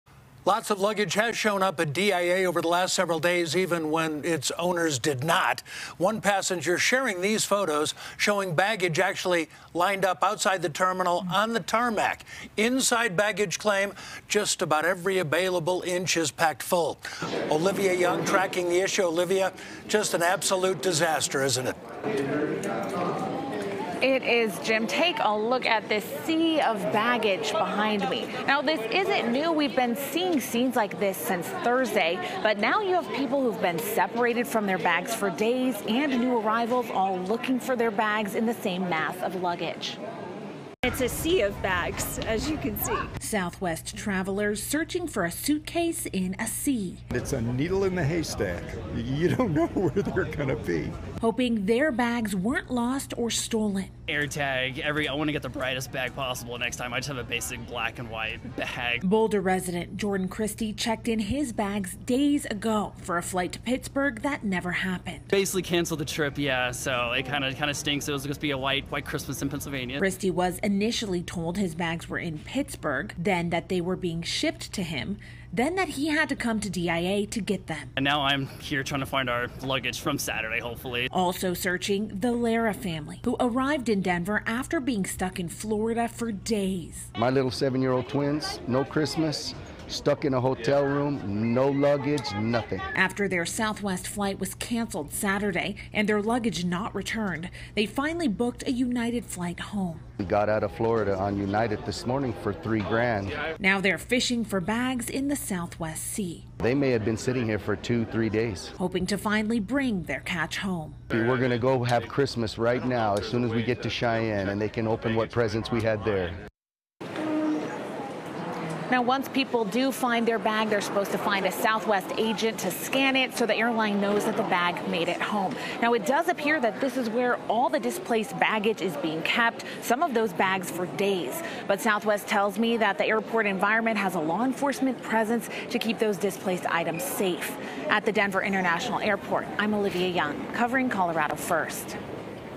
At Southwest terminal, travelers fishing in a 'sea of bags'
Denver International Airport staff say only about 1,000 passengers were stranded at DIA due to canceled flights Monday night - a sign they say means things are improving - after thousands of passengers were stranded over the weekend.